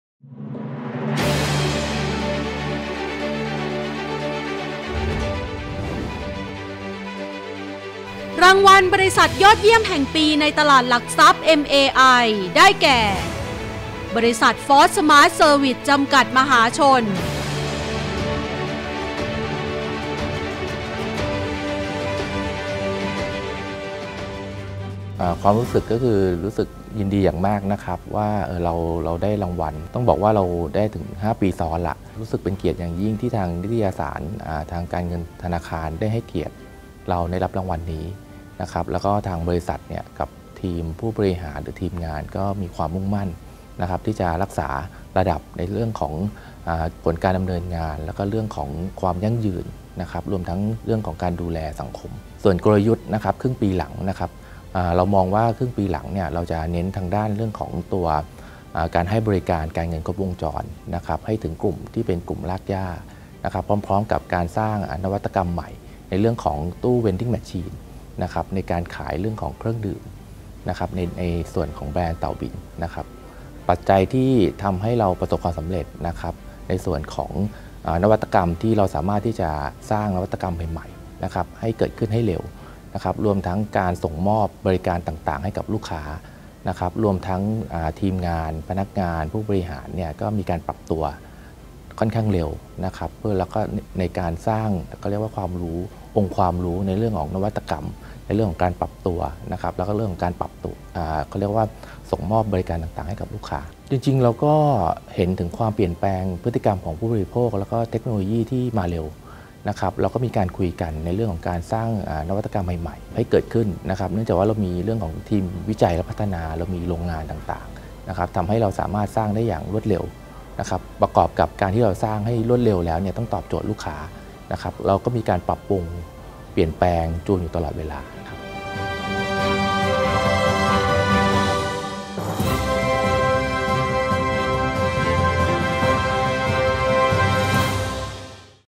รางวัลเกียรติยศ รางวัลบริษัทยอดเยี่ยมแห่งปี 2565 ในตลาดหลักทรัพย์เอ็ม เอ ไอ
รางวัลบริษัทยอดเยี่ยมแห่งปี 2565 ในตลาดหลักทรัพย์เอ็ม เอ ไอ

รับชมบทสัมภาษณ์เจาะลึกถึงความสำเร็จ พร้อมกลยุทธ์การทำธุรกิจในปี 2566 จากคุณณรงค์ศักดิ์ เลิศทรัพย์ทวี กรรมการผู้จัดการ บมจ.ฟอร์ท สมาร์ท เซอร์วิส ที่ได้รับรางวัลเกียรติยศ Money & Banking Awards 2022 จากวารสารการเงินธนาคารกันได้ในคลิปนี้